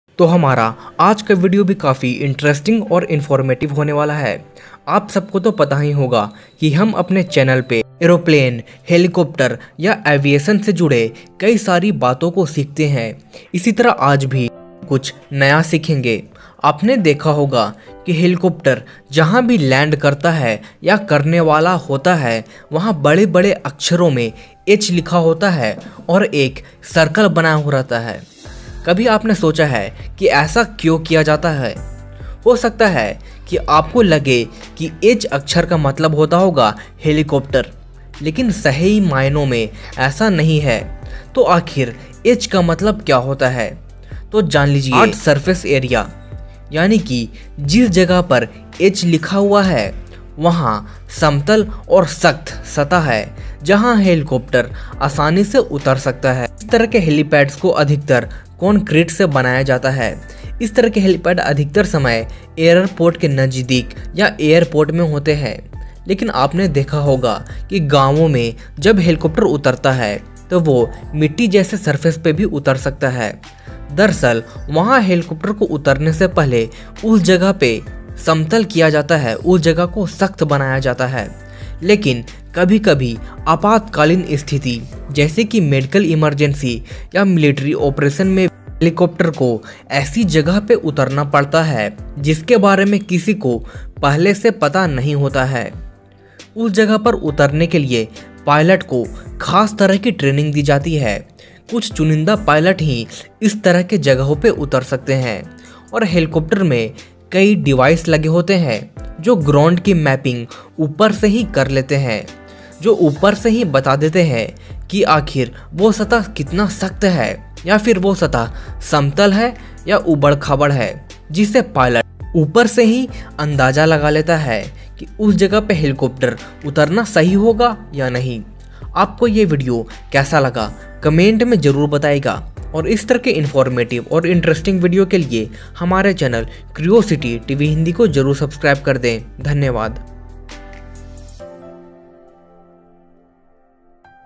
हेलीकाप्टर के लैंड करने वाले जगह पे "H" क्यों लिखा होता है | What Does "H" means on Helipad ?
This video is about "H" sign or Word Commonly Written on Helipads or place where helicopter is going to land but did you know why "H" is actually written on Helipads ?

Why does H written on Helipads
Helipads of Helicopter
Curiosity TV Hindi